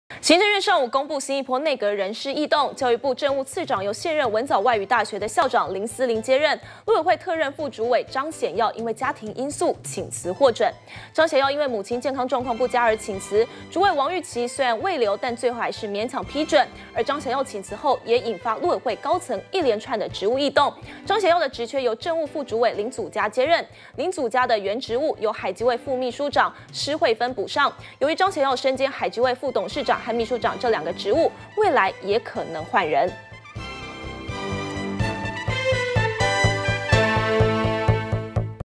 【2014.08.16】陸委會副主委張顯耀 請辭獲准 -udn tv
行政院上午公布新一波內閣人事異動。教育部政務次長由現任文藻外語大學校長林思伶接任，陸委會特任副主委張顯耀因家庭因素，請辭獲准。完整內容 , FB , Twitter , G+